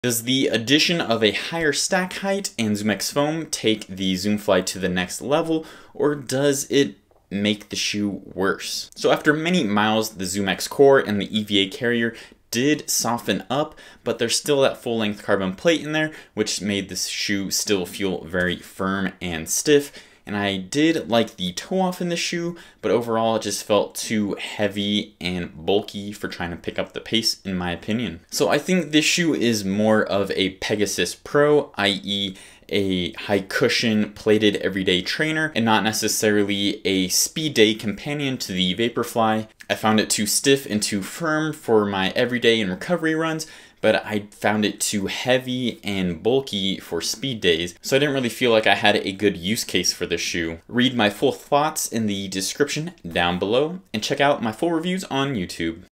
Nike Zoom Fly 5 Review
—Intro:
I loved the first two version of the Zoom Fly as they lined up very closely with the Vaporfly, but just not as premium. Unfortunately I feel like the Zoom Fly has been getting further and further away from the Vaporfly uptempo training campion it once was, and now is just a played everyday trainer.

—Upper: 🟢
I actually like the upper on this shoe. It was pretty simple, kept me locked in, and had enough padding to be comfortable on longer runs.

—Midsole: 🔴
Like many, I was hoping a ZoomX core would help make this shoe super. Unfortunately, the glued together recycled ZoomX chunks surrounded by EVA foam make this shoe feel very firm. It did soften up after some breaking in, but then the carbon plate got in the way and still made it feel too firm. I think a pebax or nylon plate would’ve been the better move here.

—Outsole: 🟢
The outsole had pretty decent grip and durability.

—Run Types:
Speed Day: 🟡
Everyday: 🔴
Long Run Day: 🟡
Recovery Day: 🔴

—Conclusion: 🔴
I was hoping this would be Nike’s version of the SC Trainer: a high stack, soft, bouncy shoe with a little pop, but to me it fell short. Have you tried this shoe? Do you like it? What are use cases for it?